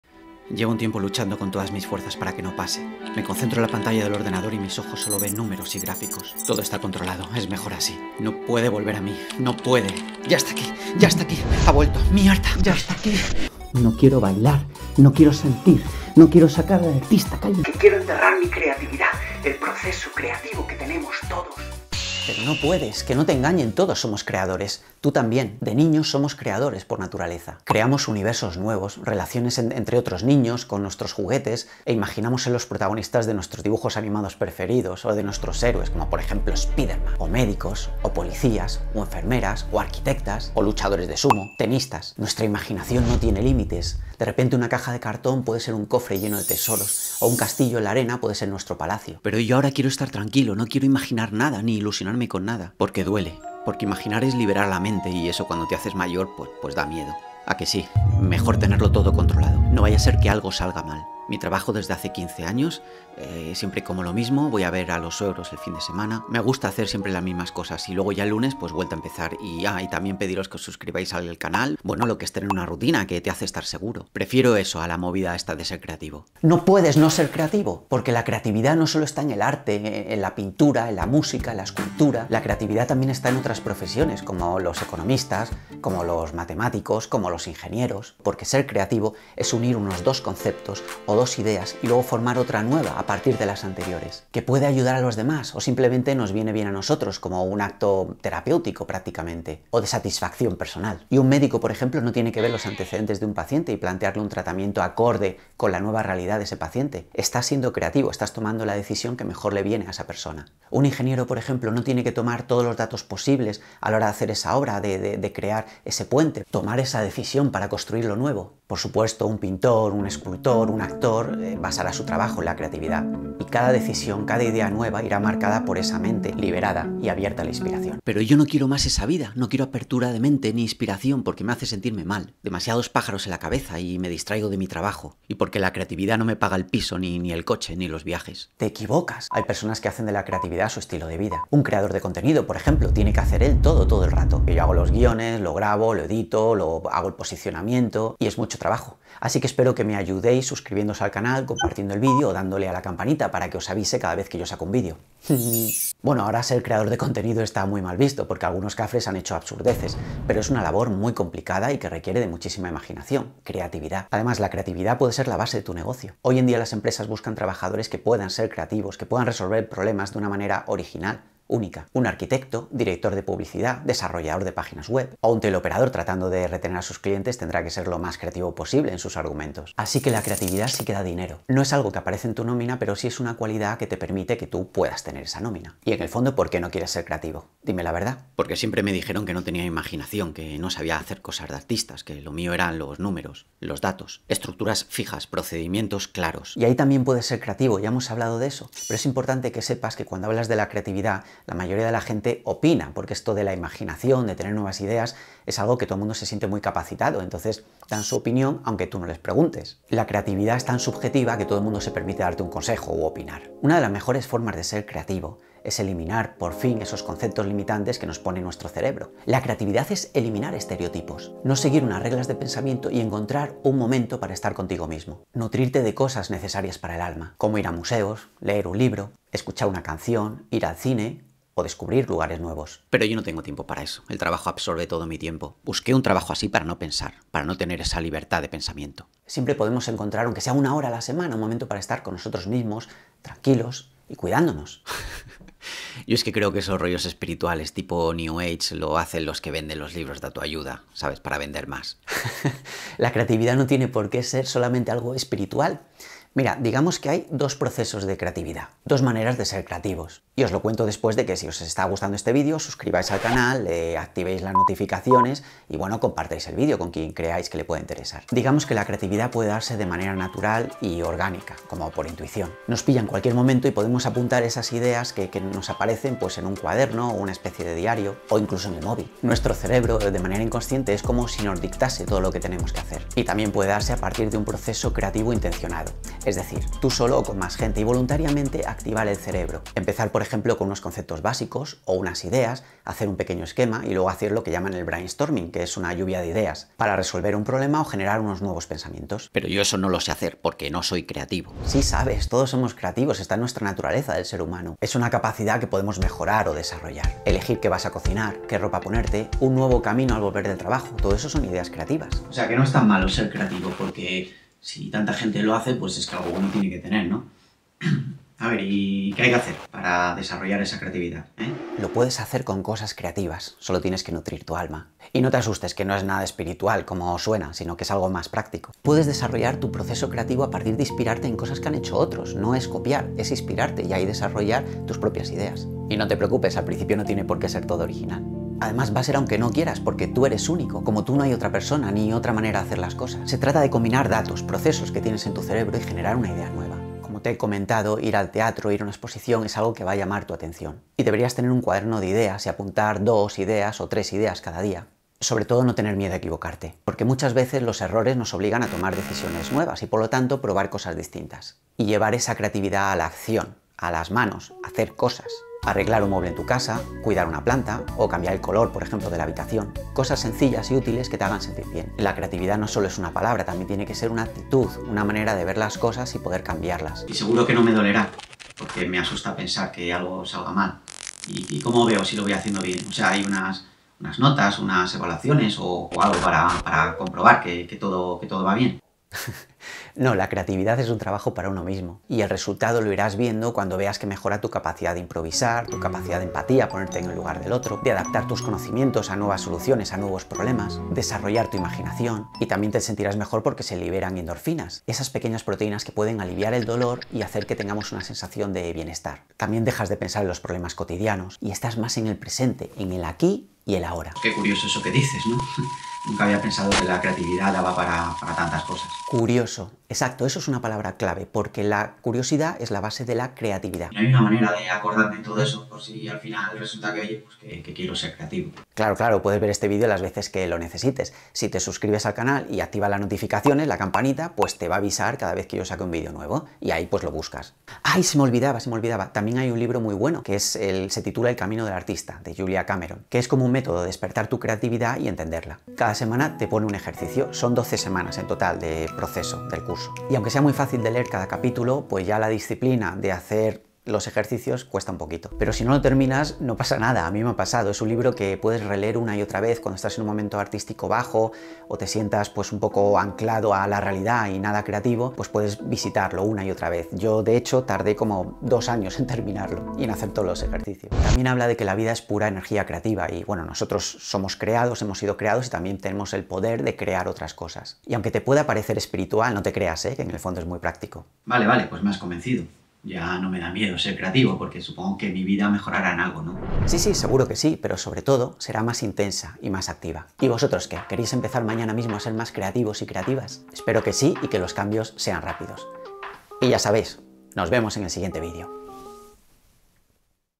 ¡ La importancia de la CREATIVIDAD! 🎊💥
¿Qué es la creatividad? ¿Todos podemos ser creativos ? ¿Te han dicho que no eres una persona creativa y te lo has creído? En este vídeo veremos cómo todos somos creativos y no podemos renunciar a ello. Y cómo desarrollar la habilidad de la creatividad cuando queramos. ¿Y para qué sirve y dónde se aplica? También hablaremos de eso, como cuándo surge ser creativo... Toma tu bol de palomitas y atiende...¡Empezamos!